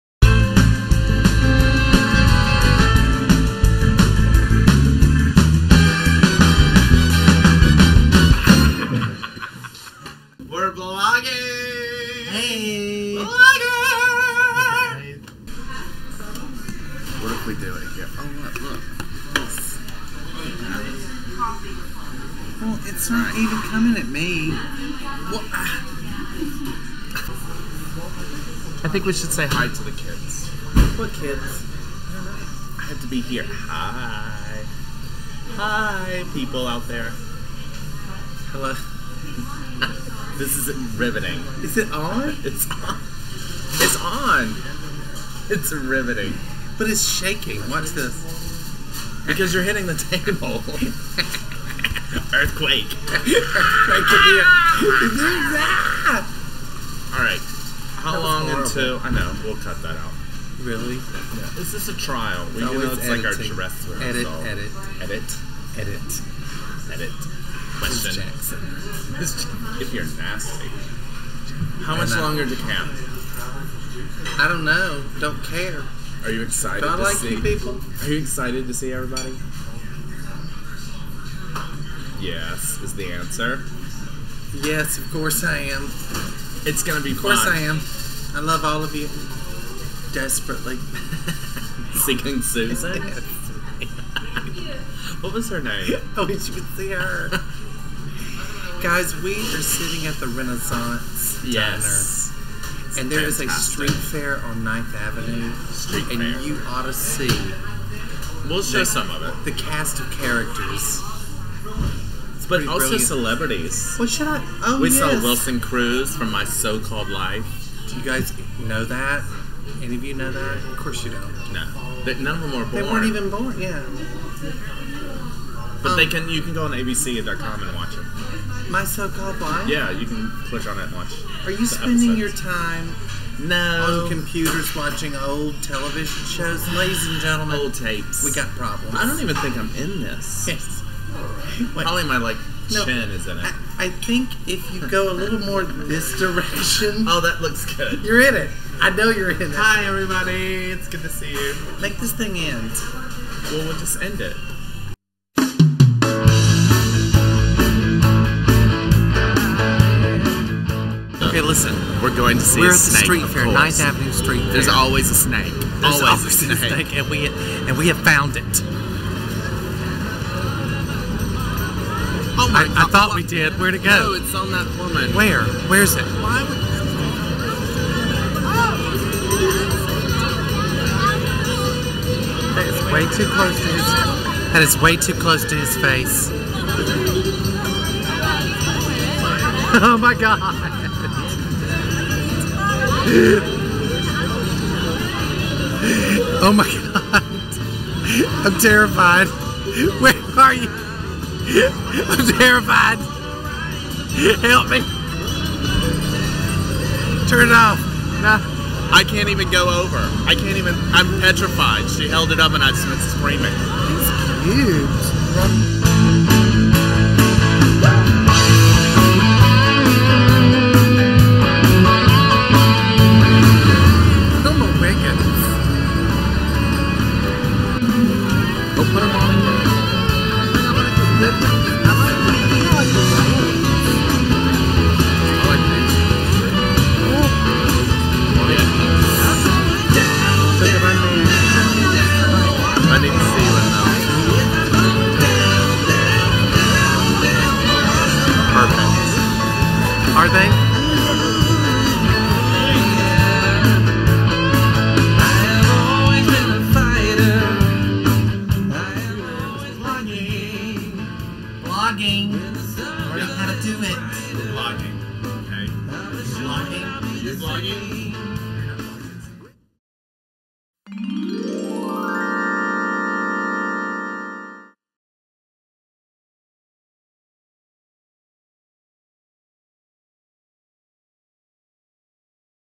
Meet me at the Stage door
Brad and Jacob at the 9th ave. Street Fair